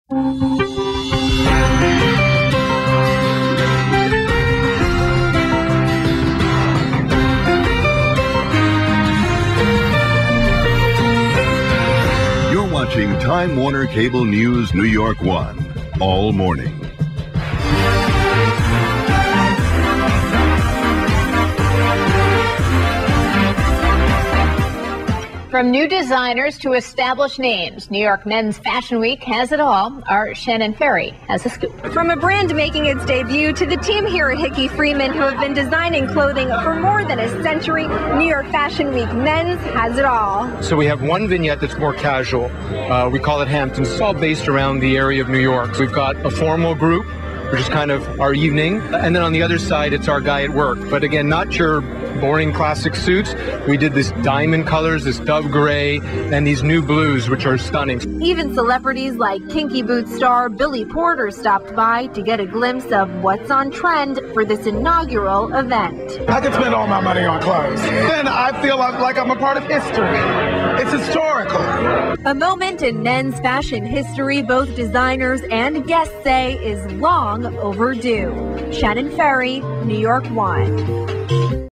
Hickey Freeman New York Fashion Week Men's Presentation on NY1
On Thursday, July 16th Hickey Freeman presented our Spring Summer 2016 collection during the first New York Fashion Week Mens at The Standard, East Village. ‪#‎NYFWM‬ ‪#‎KineticEnergy‬